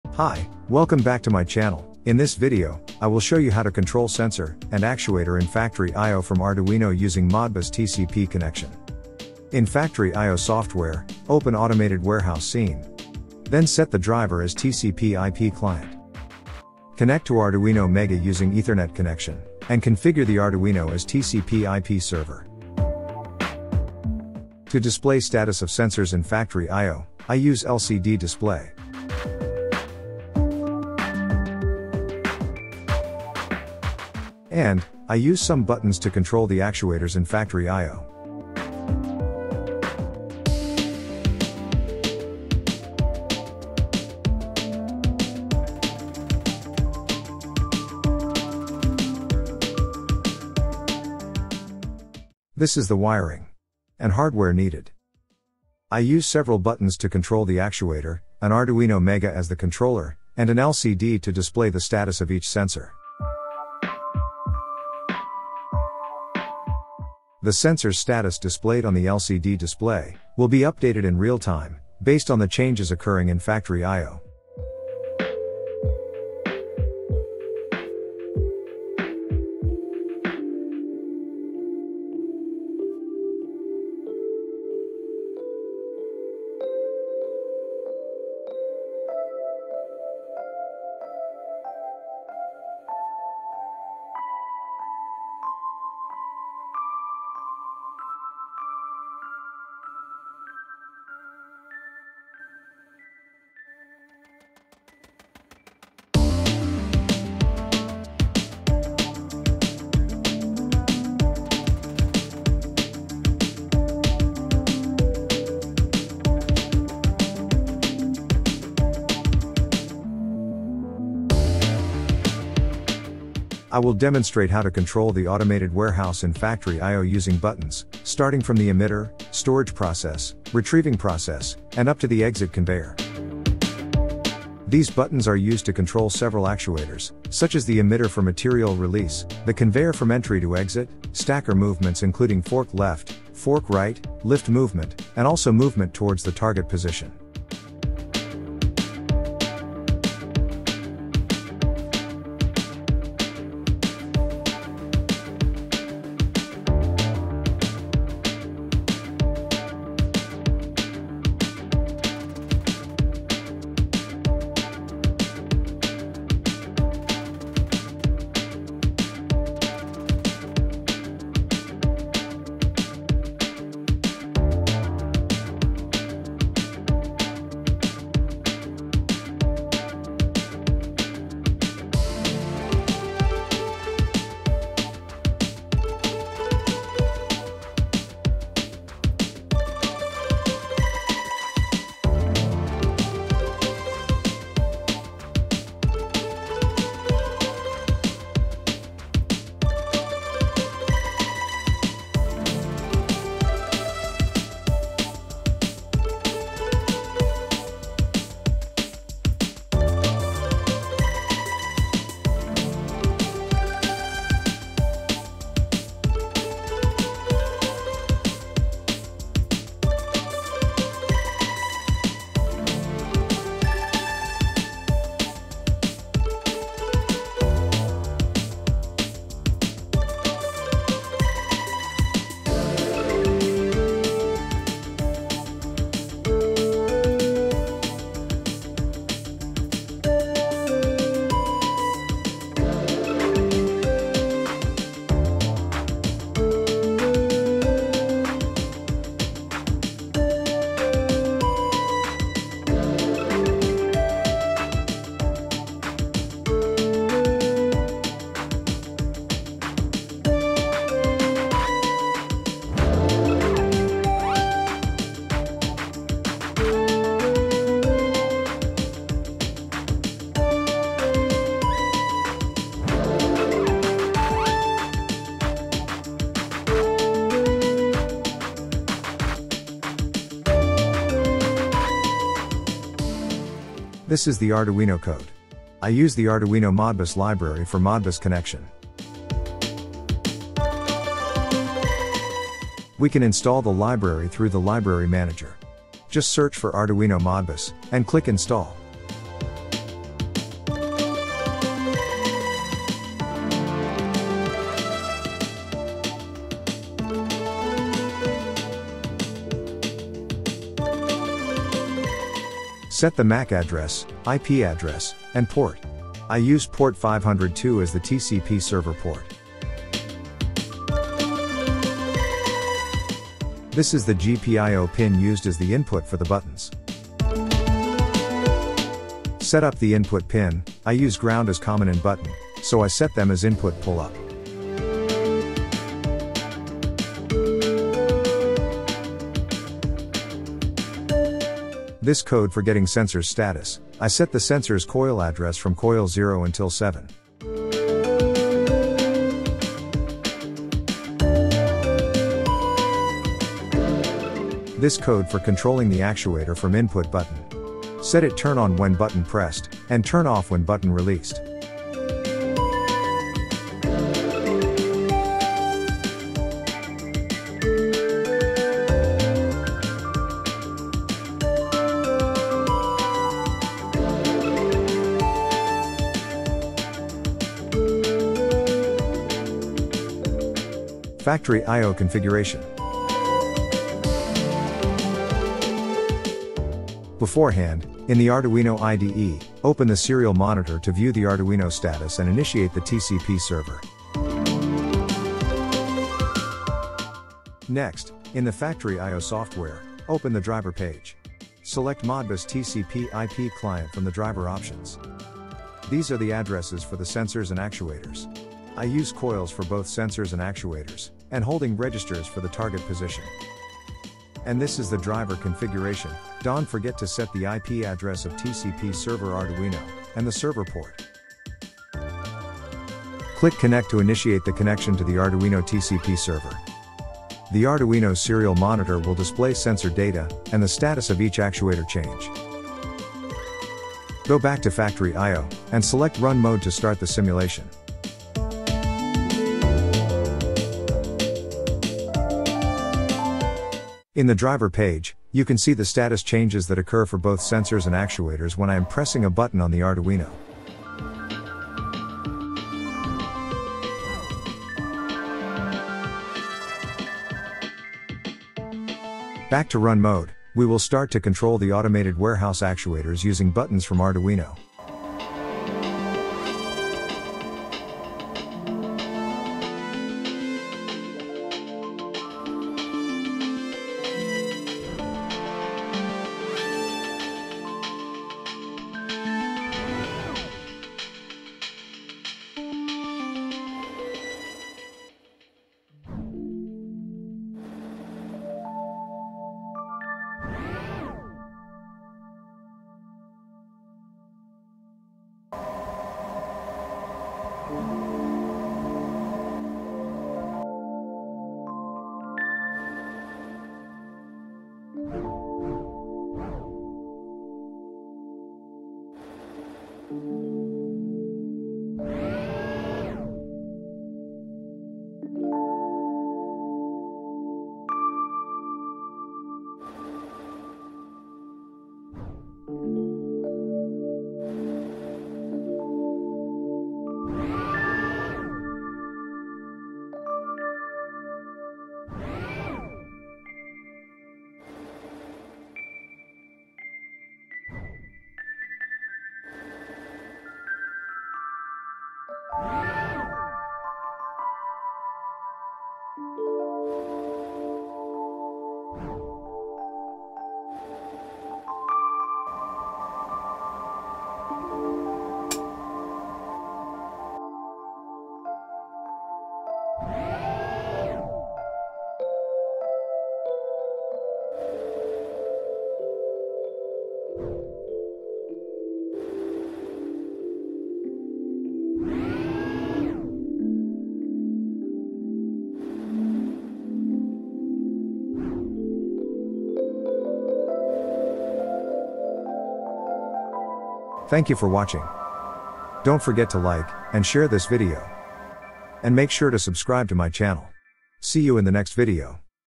Factory IO: Control Sensors and Actuator From Arduino Using Modbus TCP
Factory IO: Automated Warehouse scene
Hardware:
- Arduino Mega
- Ethernet Shield
- LCD
- Buttons
- Cable Jumper

If you find this content useful, please send me "Super Thanks"